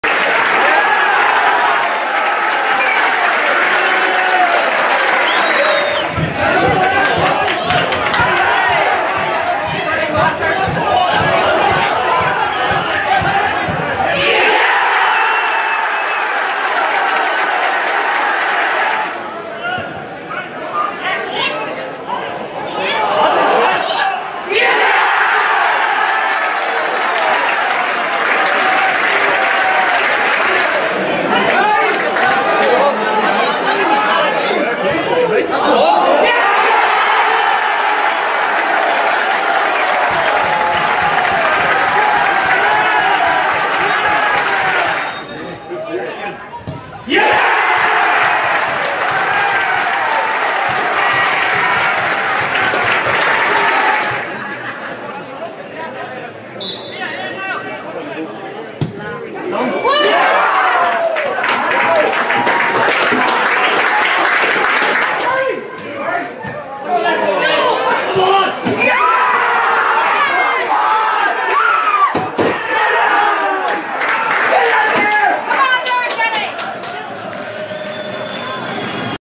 GOALS - August 2021
All 6 of our goals from August 2021. Which gets your vote for goal of the month?

Photo: Sportsfile